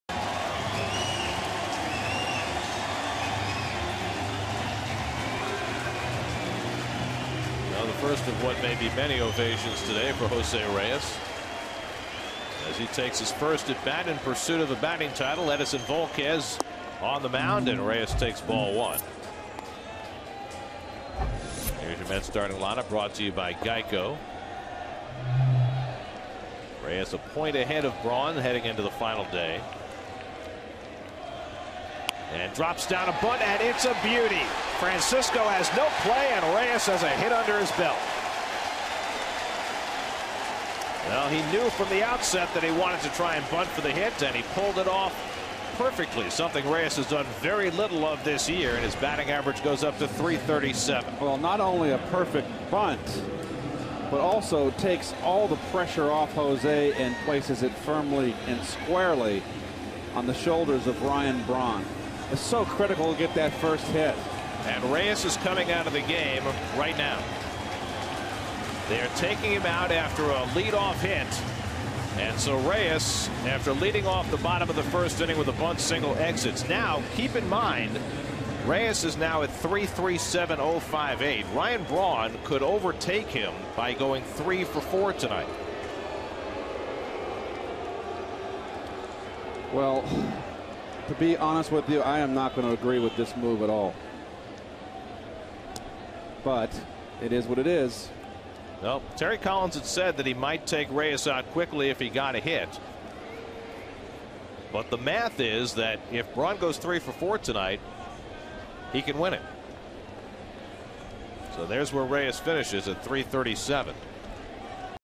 CIN@NYM: Reyes singles in final at-bat of 2011 season
9/28/11: Jose Reyes singles in his final at-bat of season winning the NL batting title, then receives an ovation from the Citi Field crowd